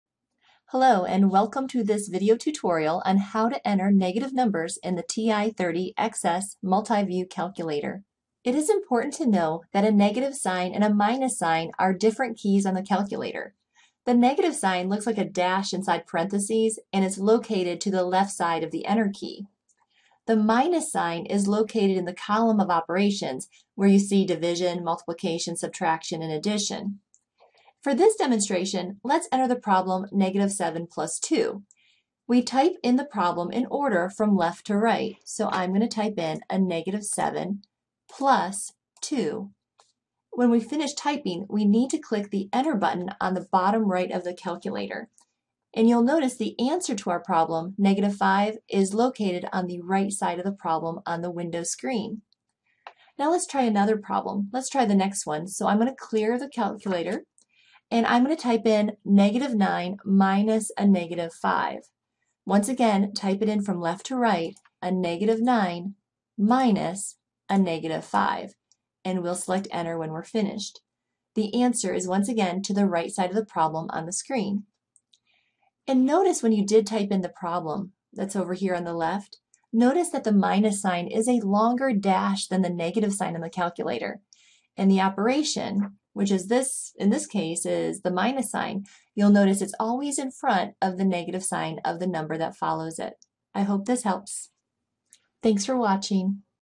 TI30XS Multiview Calculator - Negative Signs
This tutorial will show you how to enter negative signs on the TI30XS Multiview Calculator